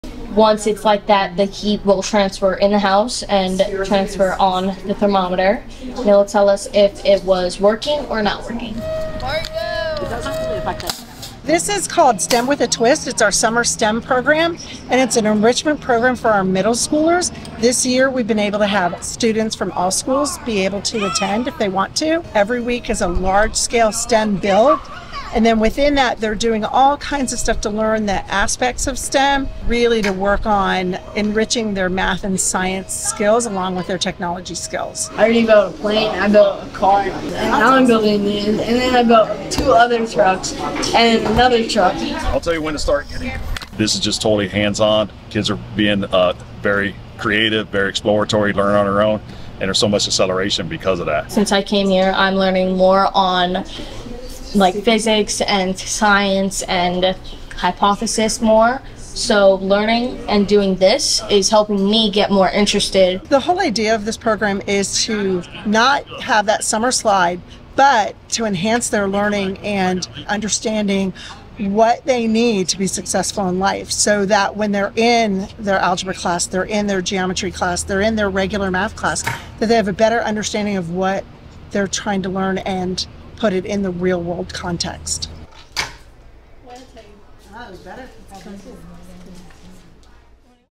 Volusia County Schools: STEM With a Twist
🌟🔬 STEM with a Twist! 🌟🔬

This summer, middle schoolers from all Volusia County Schools are diving into our enriching STEM program. Each week, students engage in large-scale STEM builds, exploring various aspects of science, technology, engineering, and math. This hands-on, creative program ensures students avoid the summer slide and enhance their skills in a real-world context. From coding to robotics, our future innovators are gearing up for success!